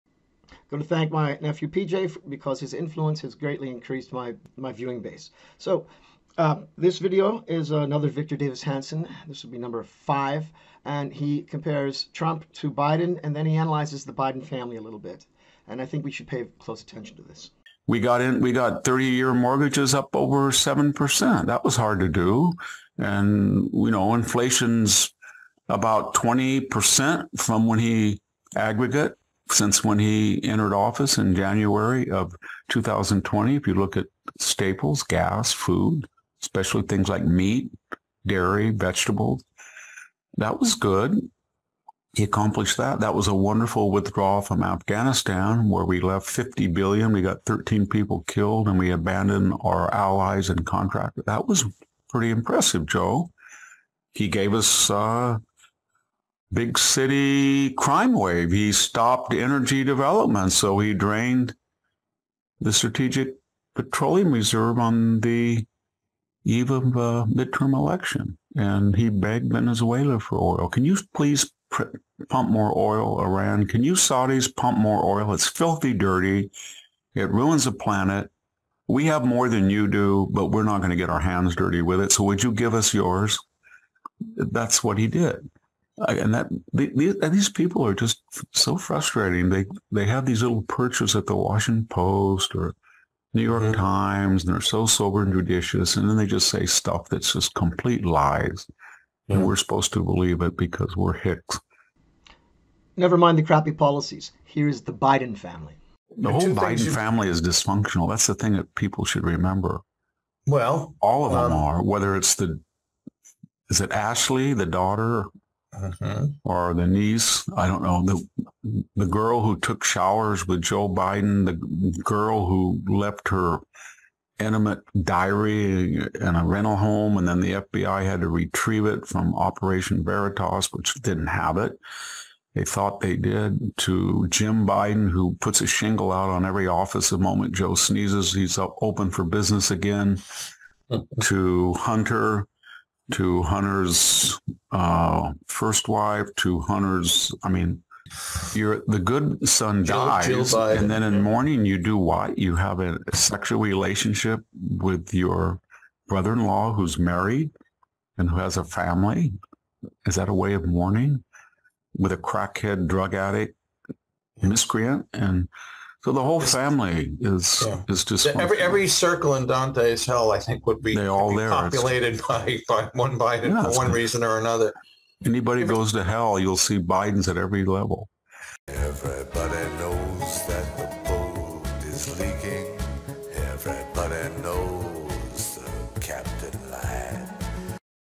Victor Davis Hanson Reaction #6
Victor Davis Hanson is an American classicist, military historian, farmer, and political commentator. He has been a commentator on modern and ancient warfare and contemporary politics for The New York Times, Wall Street Journal, National Review, The Washington Times and other media outlets.

He is a professor emeritus of Classics at California State University, Fresno, the Martin and Illie Anderson Senior Fellow in classics and military history at the Hoover Institution, and visiting professor at Hillsdale College. Hanson was awarded the National Humanities Medal in 2007 by President George W. Bush, and was a presidential appointee in 2007–2008 on the American Battle Monuments Commission.